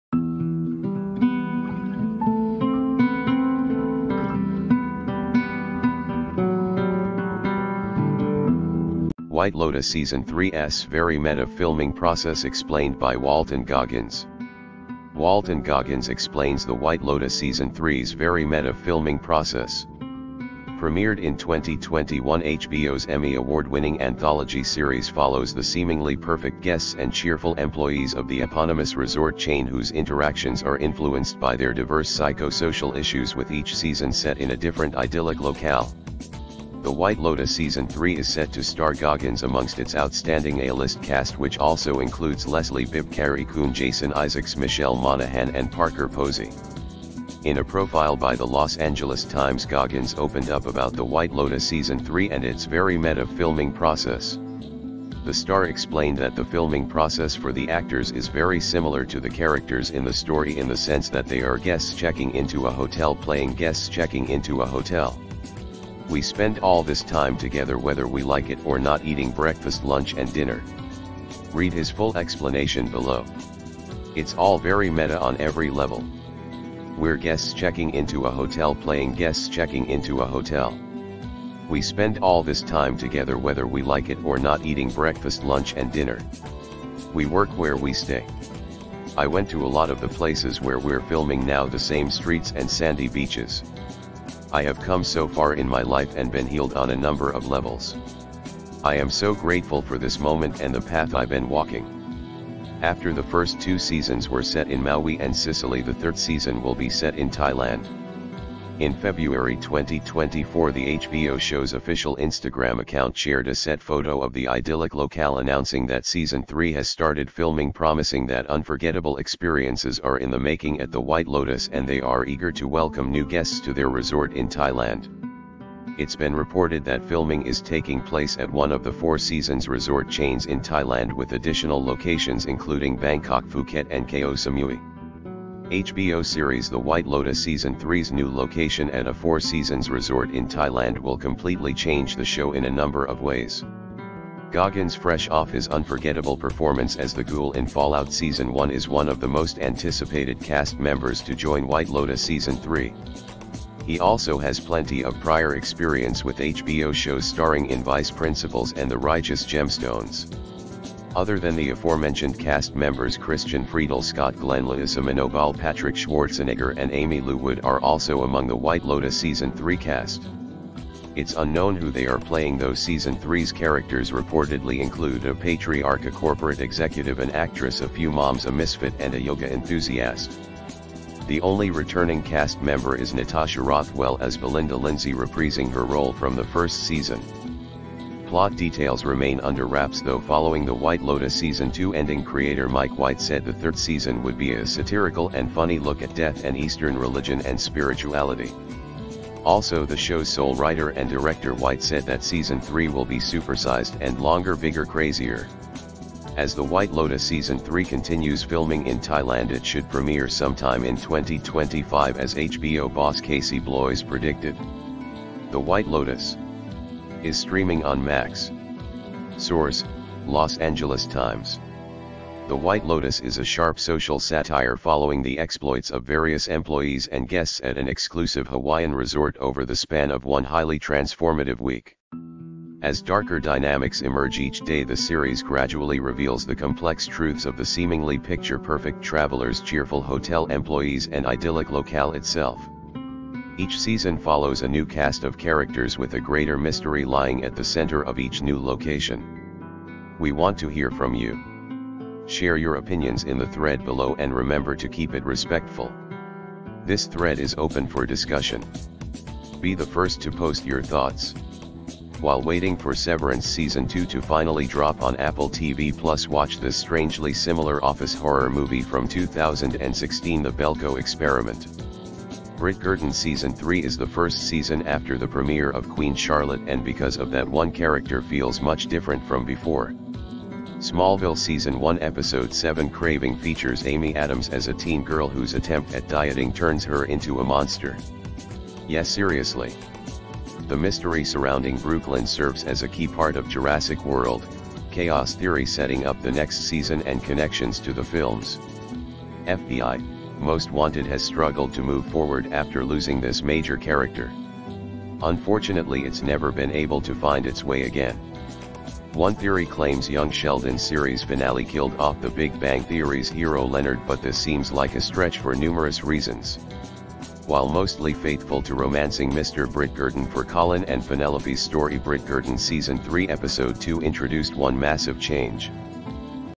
🔴White Lotus Season 3’s "Very Meta" Filming Process Explained By Walton Goggins👀
Goggins, fresh off his unforgettable performance as The Ghoul in Fallout season 1, is one of the most anticipated cast members to join White Lotus season 3. He also has plenty of prior experience with HBO shows, starring in Vice Principals and The Righteous Gemstones. Other than the aforementioned cast members, Christian Friedel, Scott Glenn, Lalisa Manobal, Patrick Schwarzenegger, and Aimee Lou Wood are also among The White Lotus season 3 cast. It's unknown who they are playing, though season 3's characters reportedly include "a patriarch, a corporate executive, an actress, a few moms, a misfit, and a yoga enthusiast."